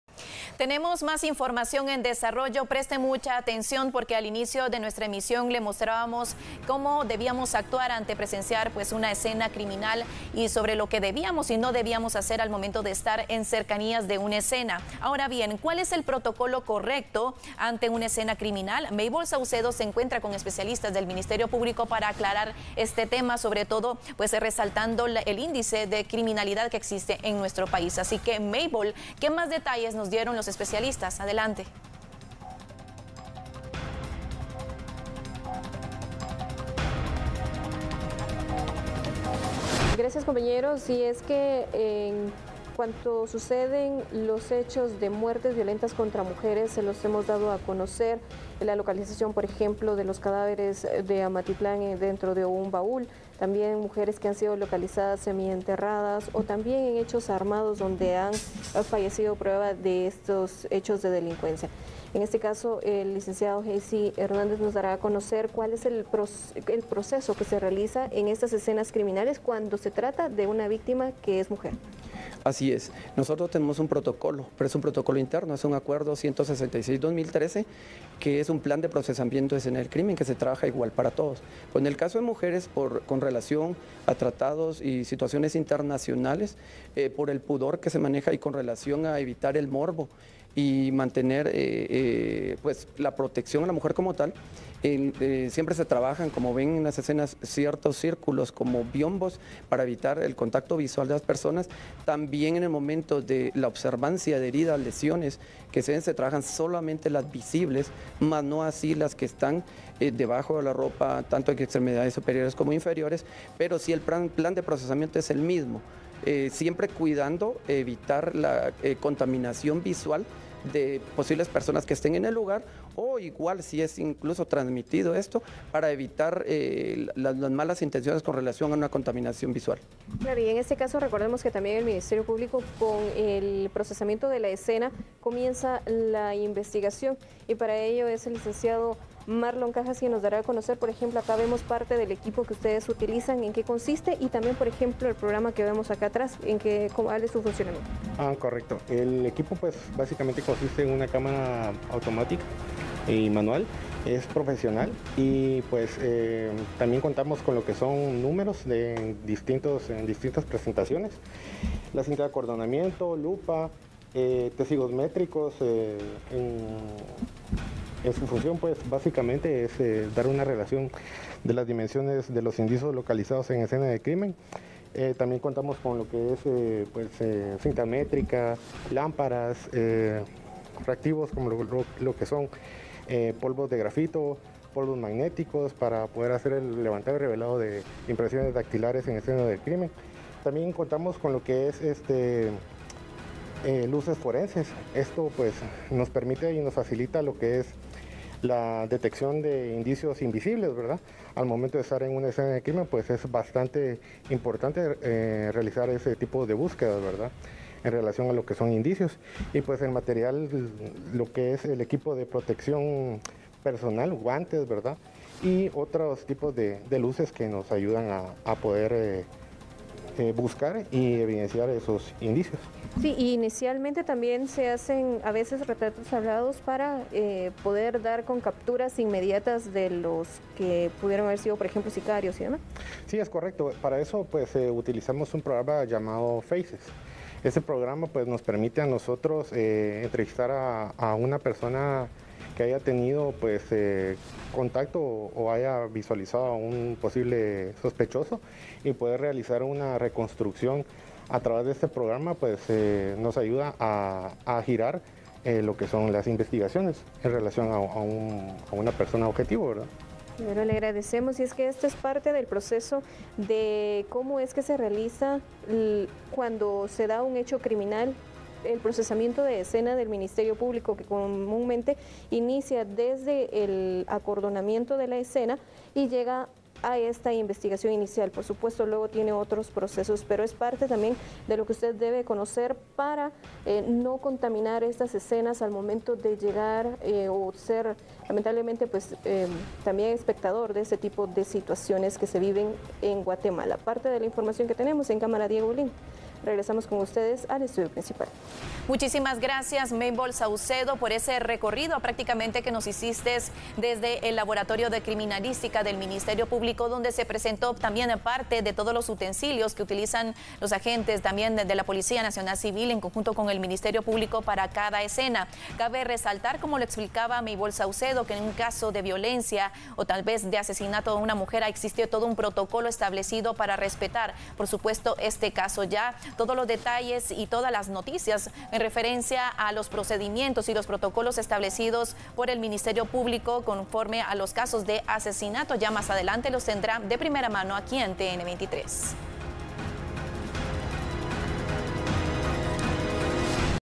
¿Cuál es el protocolo correcto ante una escena criminal?
Se debe proteger el lugar para no contaminar la evidencia

Conversamos con especialistas del MP sobre el protocolo correcto a seguir en una escena del crimen.